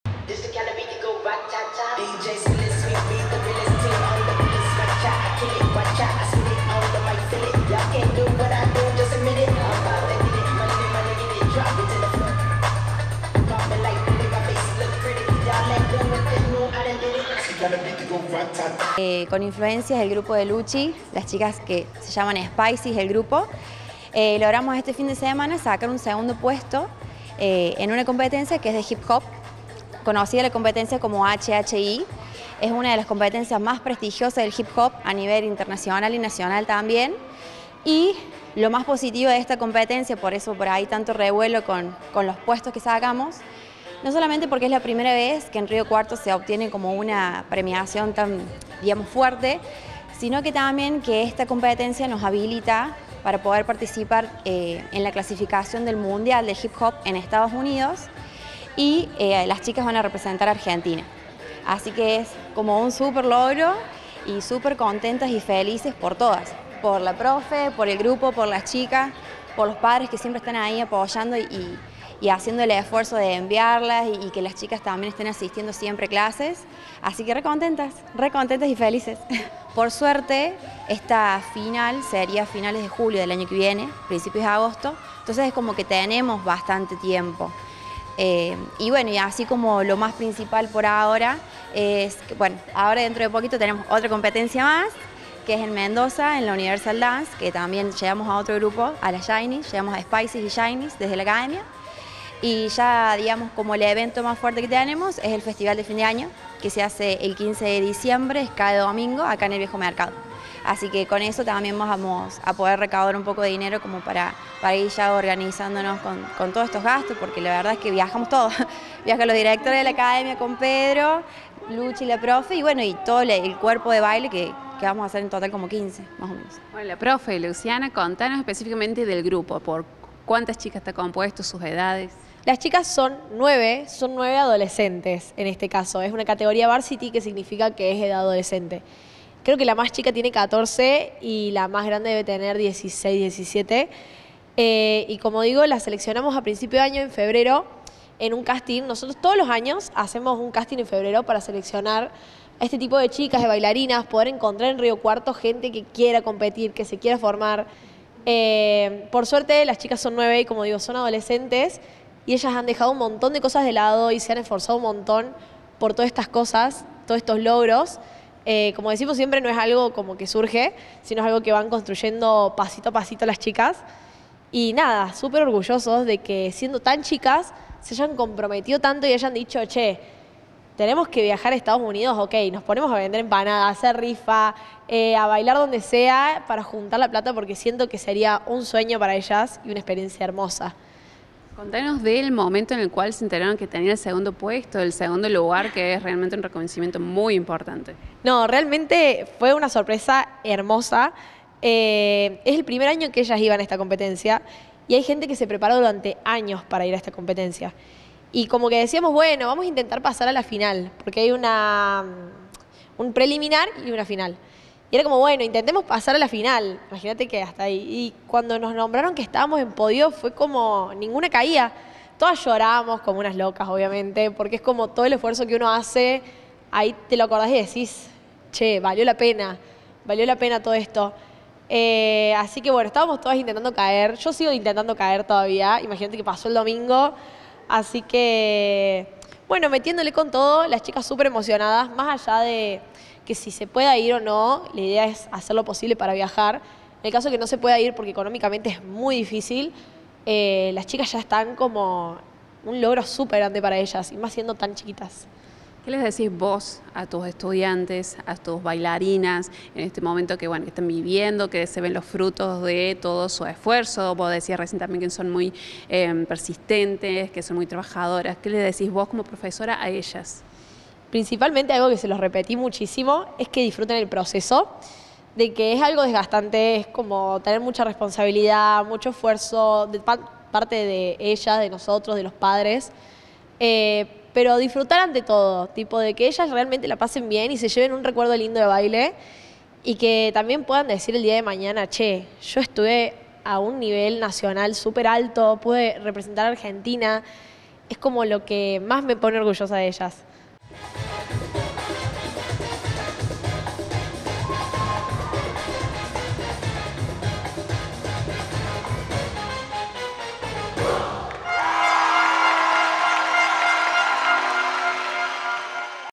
ACADEMIA INFLUENCIAS EN PODIO EN HHI
La competencia de Hip Hop Internacional se llevó adelante este domingo en Buenos Aires, una instancia nacional. Río Cuarto tuvo el segundo puesto. A mediados del año próximo sueñan con competir internacionalmente en Estados Unidos. Dialogamos con Dayana Garay, directora de Influencias; y Luciana Revol, profesora.